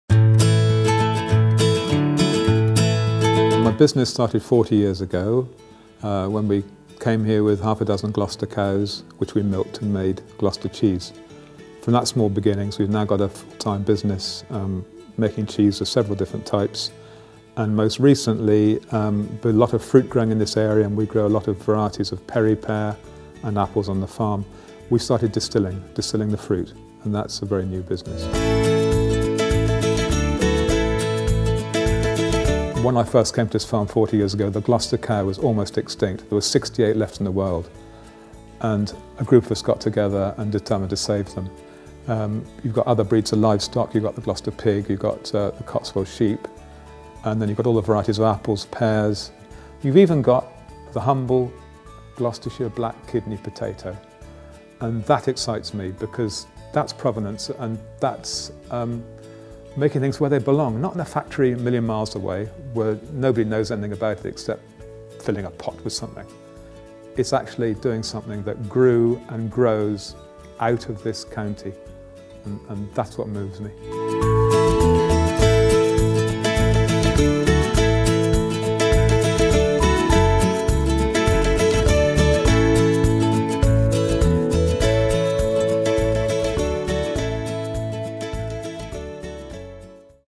Charles Martell and Son
This was one of three films made for the outstanding contribution to food catagory of the Gloucestershire media Taste of Gloucestershire Food and Farming awards 2012.